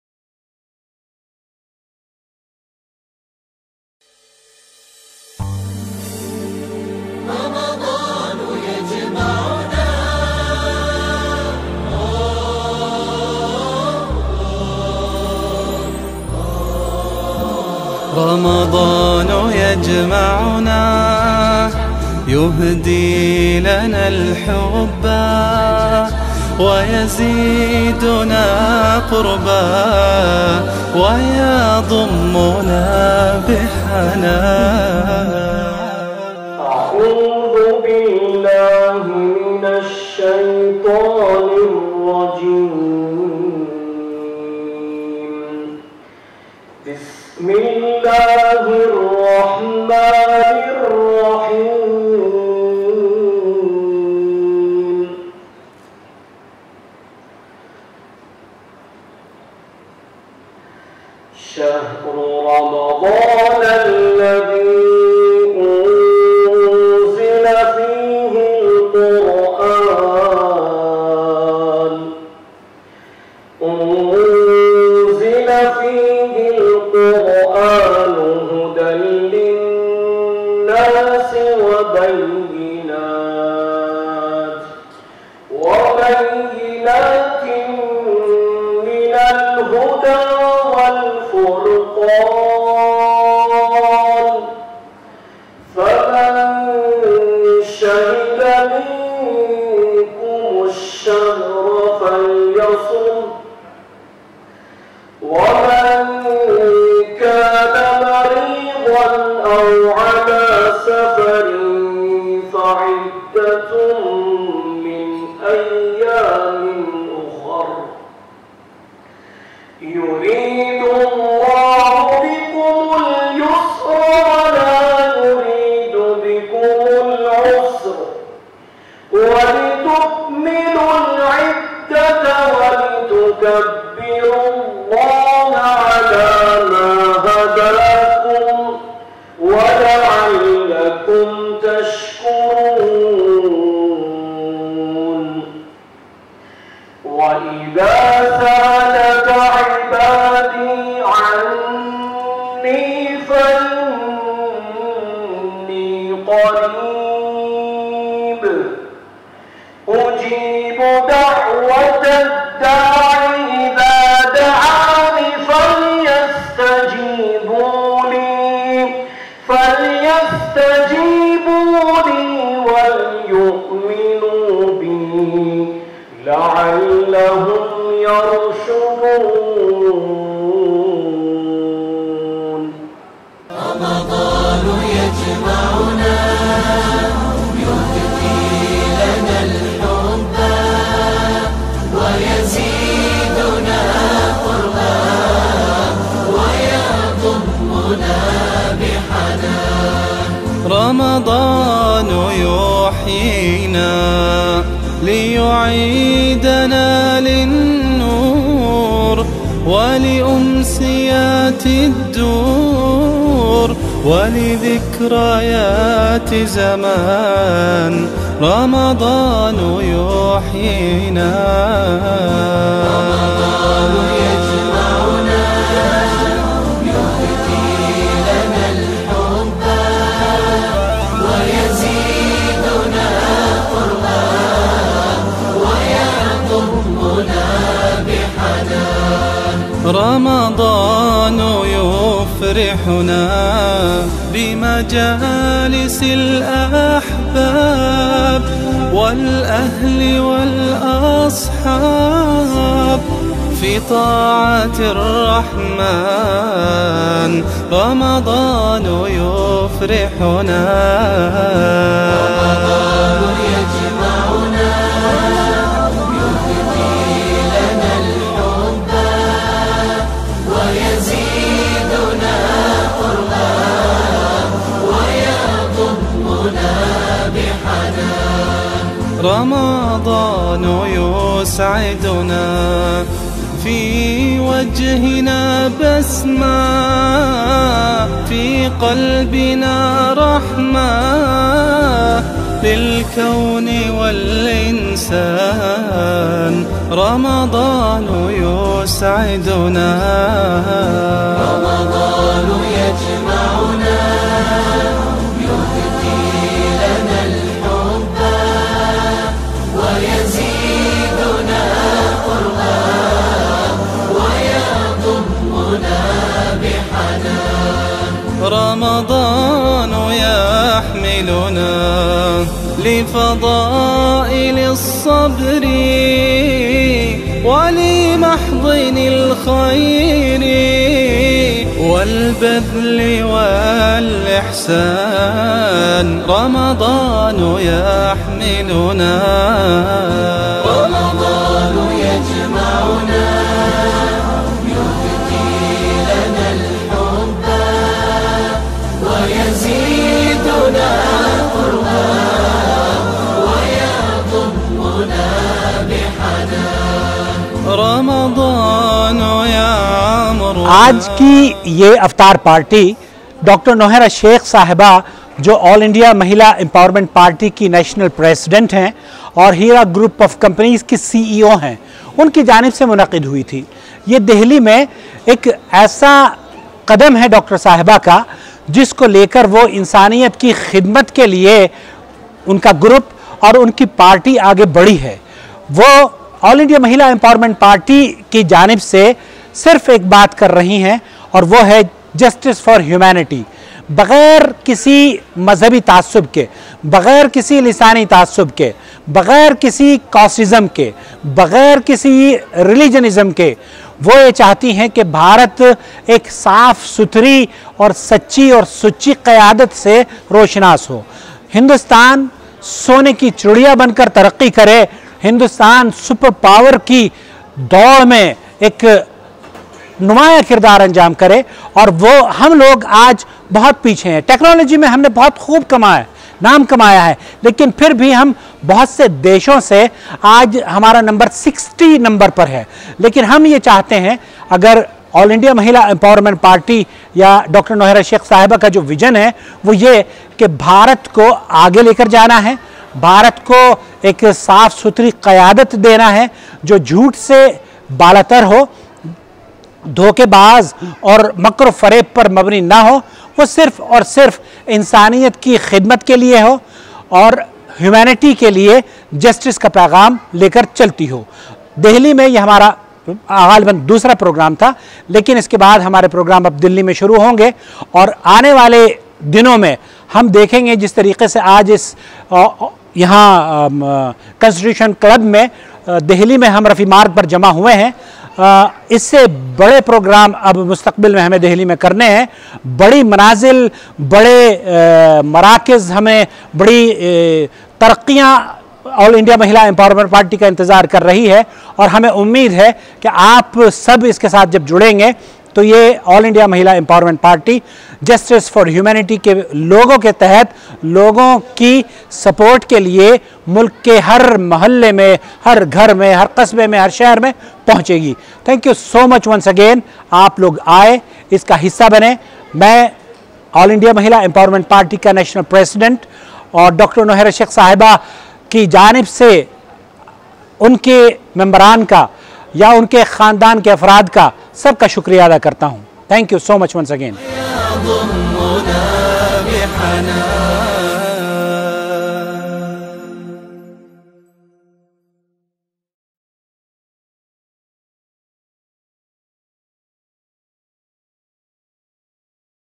Heera Group & MEP Delhi State Iftar Get Together on 30 May 2018
An Iftaar Get Together was organized in Delhi on May 30, 2018 at Constitution Club, Rafi Marg, New Delhi by Dr.  Nowhera Shaik, CEO of Heera Group of Companies Heera Golds and National President of All India Mahila Empowerment Party (MEP) .
Please have a look on the video and join us to pray for Dr. Nowhera Shaik and her organization.  May Allah Bestow His Mercy on her and protect her from all kinds of evils.  Ameen

 أفطر عندكم الصائمون ، وأكل طعامكم الأبرار ، وصلّت " وفي رواية وتنزّلت " عليكم الملائكة